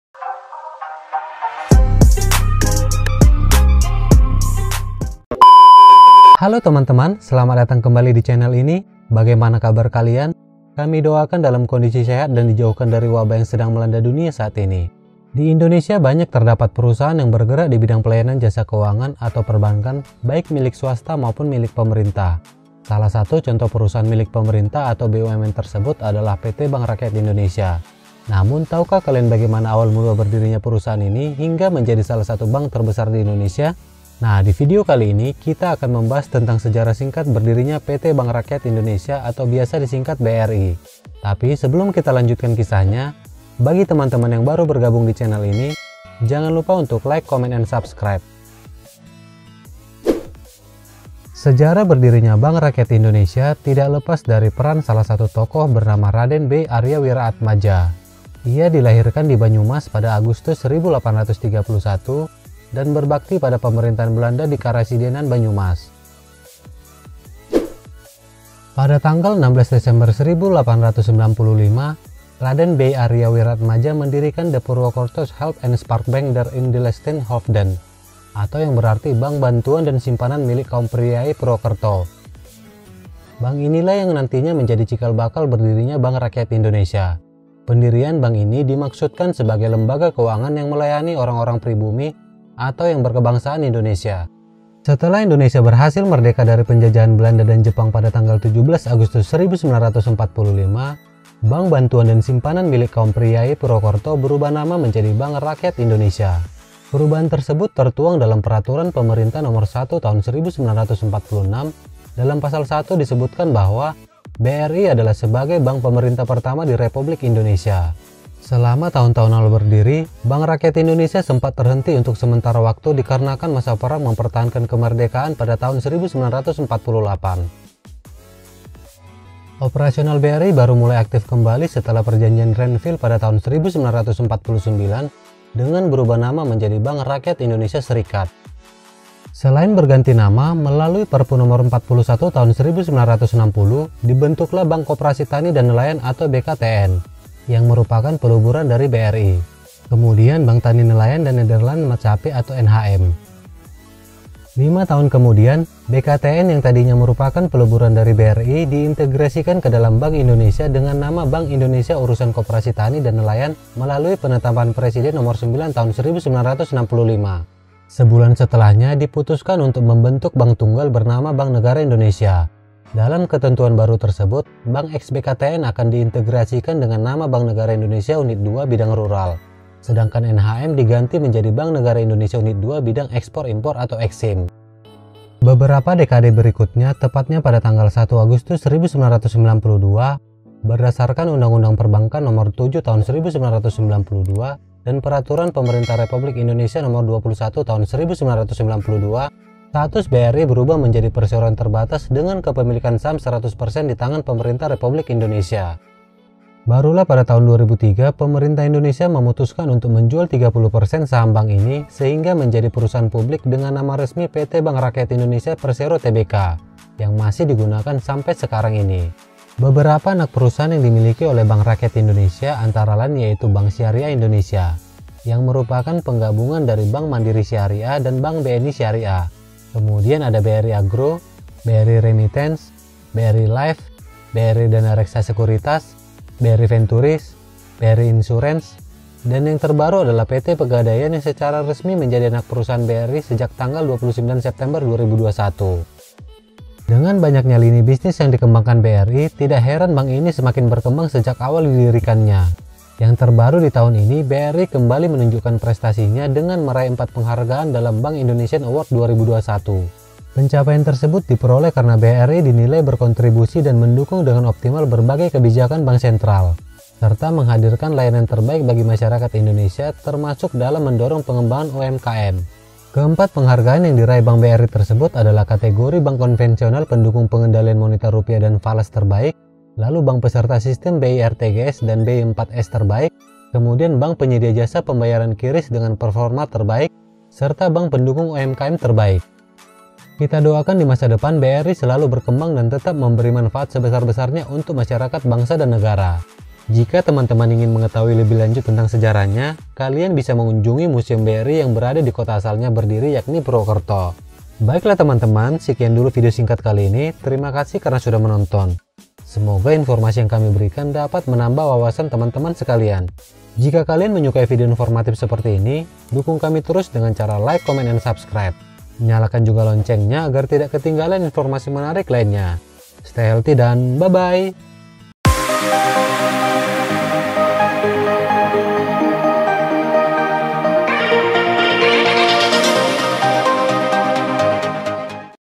Sejarah BRI, Bank Tertua Di Indonesia
Dividio Kali ini kita akan membahas tentang Sejarah BRI, Bank Pertama Indonesia.
Sejarah berdirinya Bank Rakyat Indonesia (BRI) tidak lepas dari peran salah satu tokoh bernama Raden Bei Aria Wiraatmadja. Ia dilahirkan di Banyumas pada Agustus 1831 dan berbakti pada pemerintahan Belanda di Karesidenan Banyumas.
Pada Tanggal 16 Desember 1895, Raden Bei Aria Wirjaatmadja mendirikan De Poerwokertosche Hulp en Spaarbank der Inlandsche Hoofden atau yang berarti Bank Bantuan dan Simpanan Milik Kaum Priyayi Purwekerto. Bank inilah yang nantinya menjadi cikal bakal berdirinya Bank Rakyat Indonesia. Pendirian Bank ini dimaksudkan sebagai lembaga keuangan yang melayani orang-orang pribumi atau yang berkebangsaan Indonesia. Untuk informasi selanjutnya kalian bisa cek dividio kami.